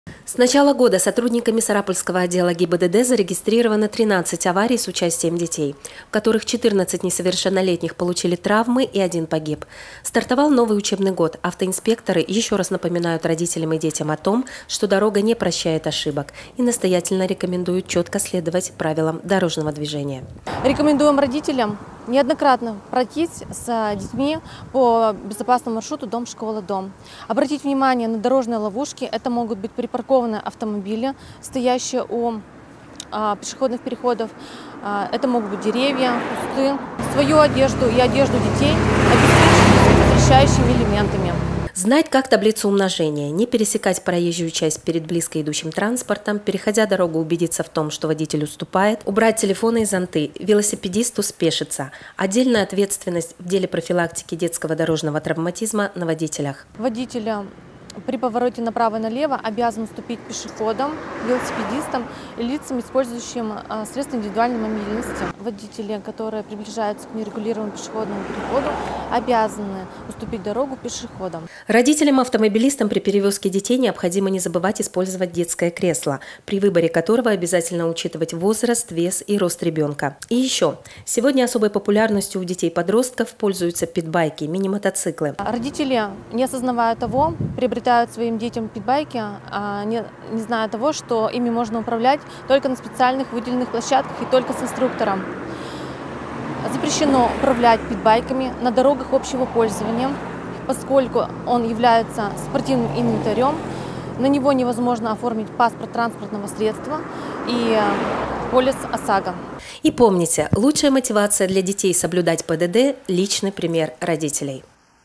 «Внимание - дети!»
В Удмуртии проходит профилактическое мероприятие «Внимание - дети!», цель которого – усилить профилактику детского дорожно-транспортного травматизма с началом нового учебного года. В период проведения акции автоинспекторы напоминают о необходимости соблюдения правил дорожного движения и обеспечения безопасности детей на дороге на родительских собраниях, торжественных линейках и классных часах в детских садах и школах, проводят профилактические рейды.